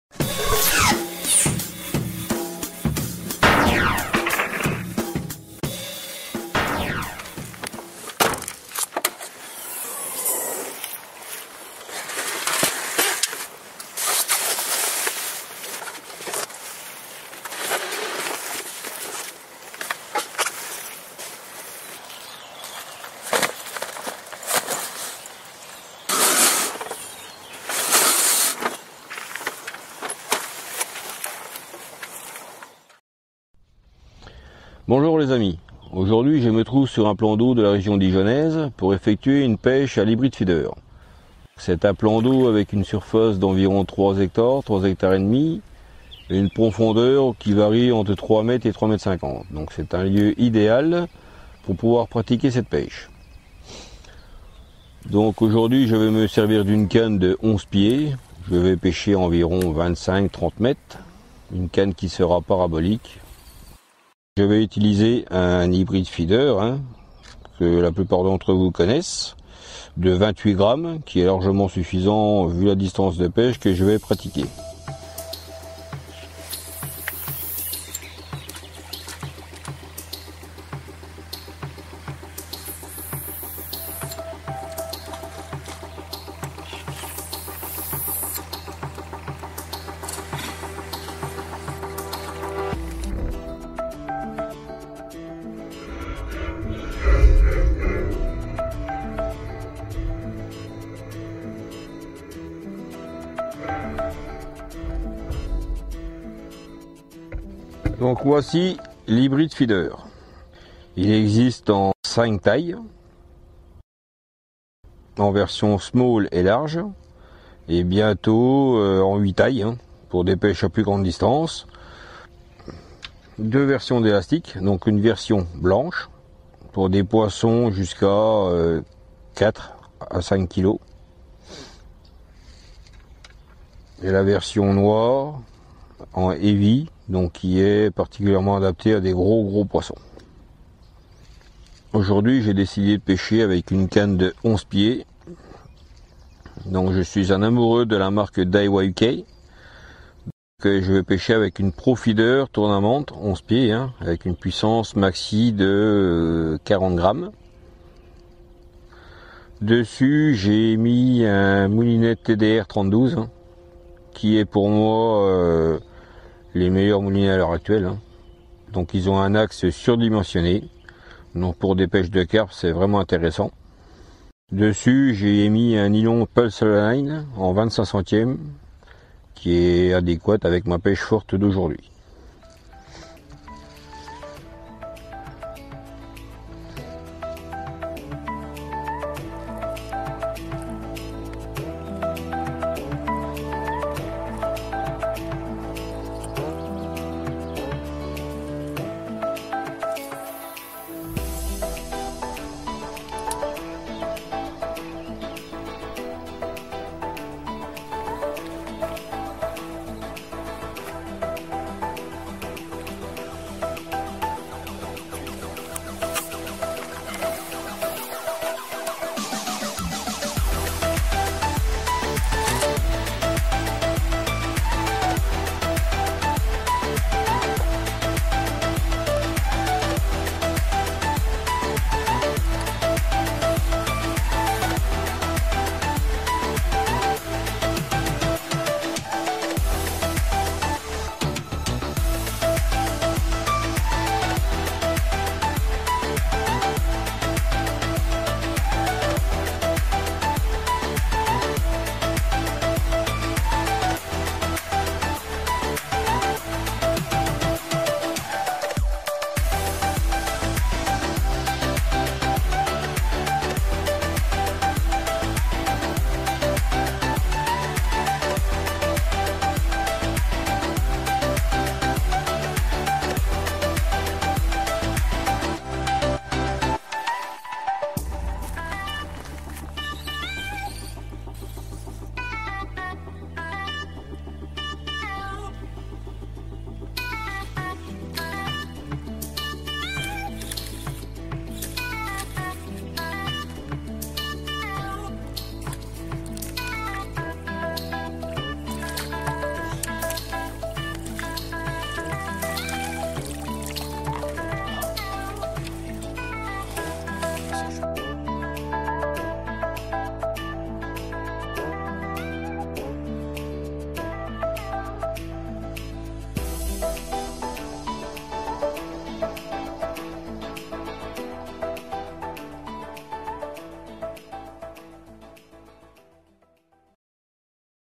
Hybrid Feeder Guru par Jérôme.
Cette fois-ci, Jérôme Ravelet, de la région dijonnaise, nous fait une démonstration avec l'hybrid feeder Guru.
Explications, astuces... encore un grand merci à Jérôme.